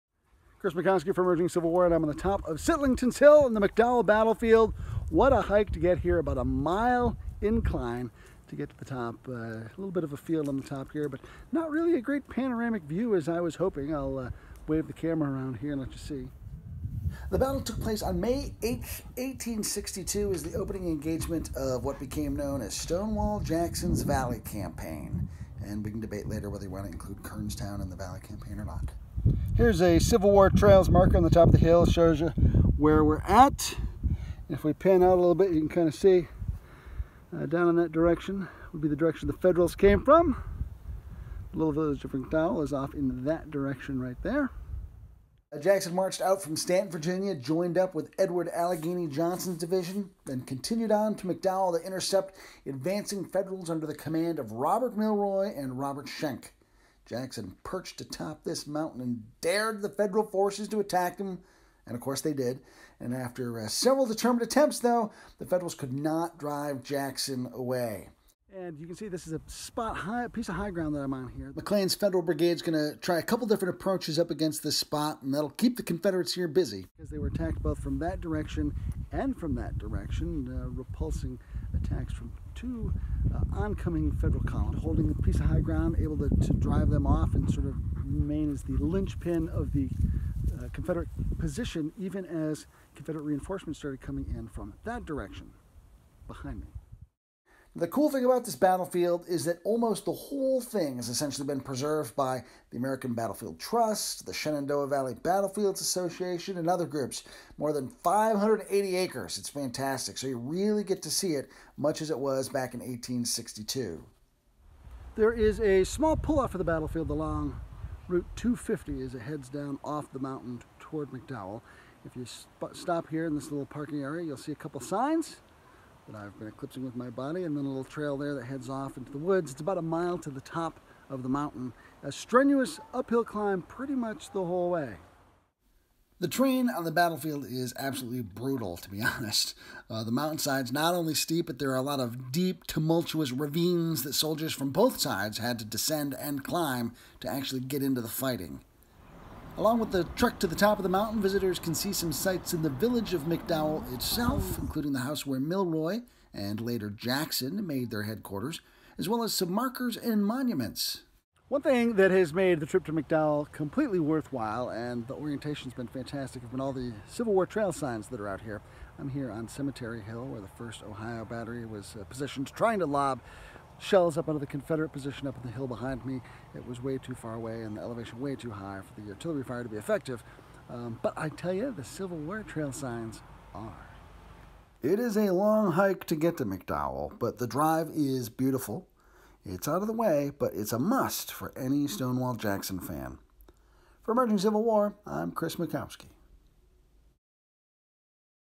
Battle of McDowell
Emerging Civil War's Chris Mackowski hikes to the top of the McDowell battlefield for the May 8 anniversary. (with thanks to the American Battlefield Trust for the map!)